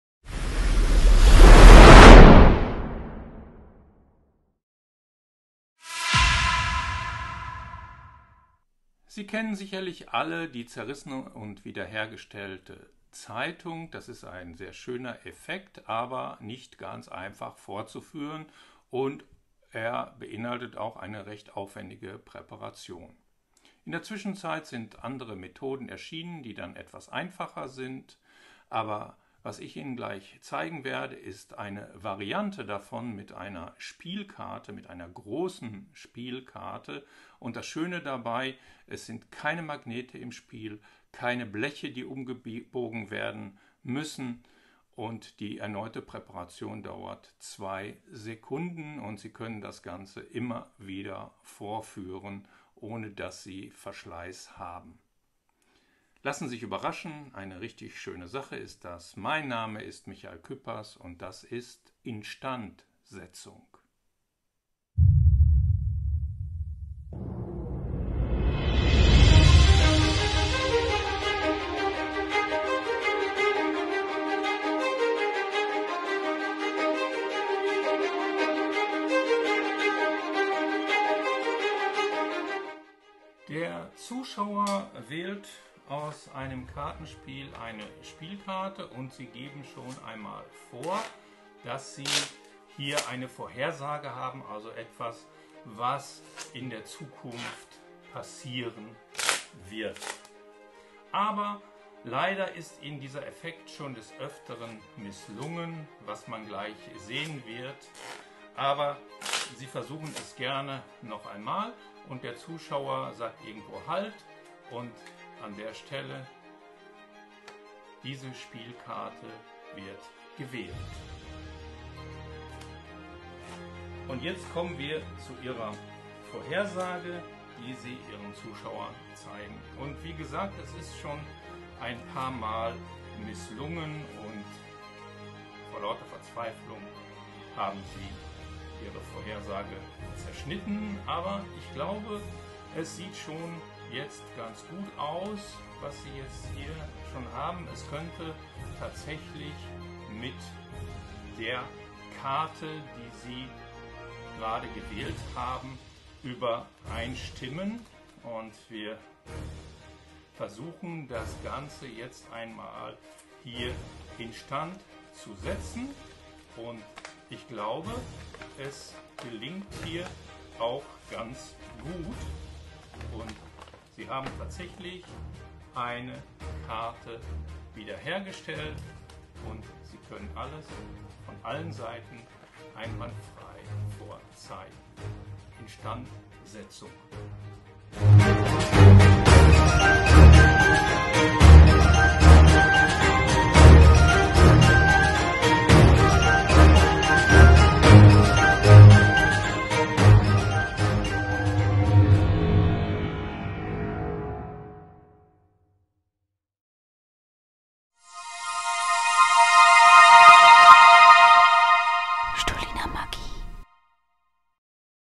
Instandsetzung by Fokx Magic
Dieser Artikel ist ab sofort in unserem Online-Shop erhältlich:

Restaurieren Sie eine zerrissene Riesenkarte innerhalb von Sekunden!
Vielen ist sicherlich das Zeitungszerreißen bekannt. Eine wirklich saubere Vorführung sieht man allerdings nur selten.

Hier bieten wir Ihnen eine Variante, die Sie mit Sicherheit vorführen werden, da sie sehr visuell und sehr einfach vorzuführen ist. Es kann nichts schiefgehen.

Nachdem der Zuschauer eine Spielkarte gewählt hat, präsentieren Sie stolz Ihre Vorhersage. Diese besteht allerdings aus lauter Einzelteilen einer großen Spielkarte. Die Papierstücke werden vorgezeigt und nach und nach zusammen gelegt. Ohne eine verdächtige Bewegung und Fummelei falten Sie die Papierstücke auseinander, bis Sie eine riesige Spielkarte in der Hand halten. Alles kann von beiden Seiten gezeigt werden. Sie brauchen nichts heimlich beiseite zu bringen. Keine Magnete, Blechteile oder unsichere Handhabungen.

Sekundenschneller Reset und immer wieder vorzuführen. Kein Verschleiß und keine Bastelarbeit. Der Trailer zeigt Ihnen den genauen Ablauf und eine Videoanleitung in deutscher Sprache genau wie es geht. INSTAND SETZUNG ist ein Highlight, mit dem Sie reichlich Applaus ernten werden.